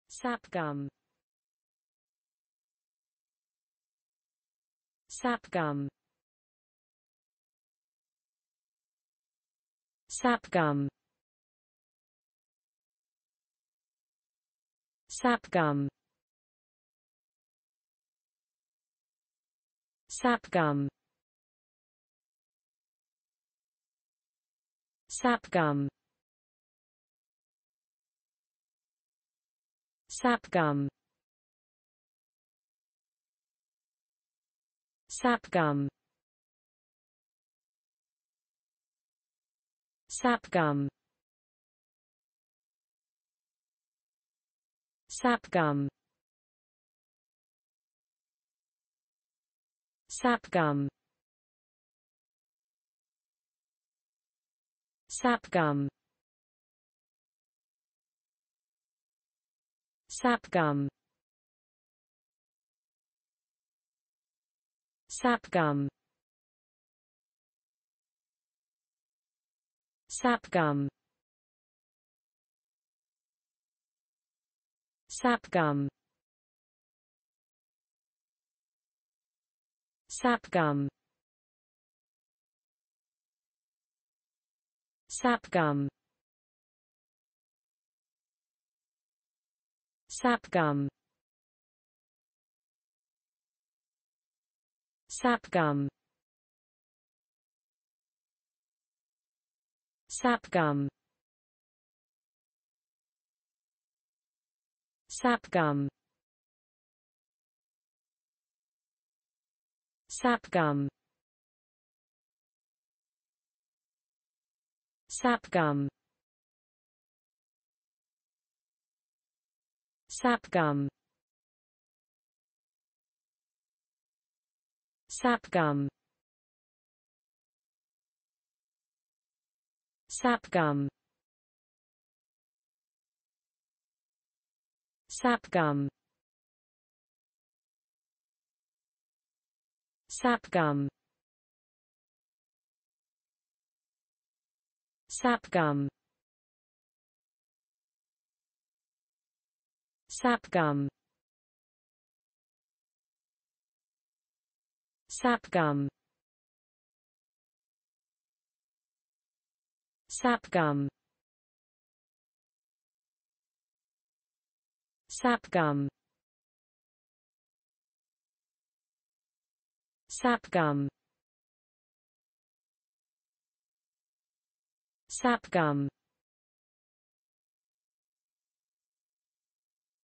Sap Gum. How to pronounce Sap Gum in english (correct!). Start with S. Learn from me.
Follow me!Learn from me how to pronounce the english word Sap Gum.This video shows you how to pronounce the English word Sap Gum .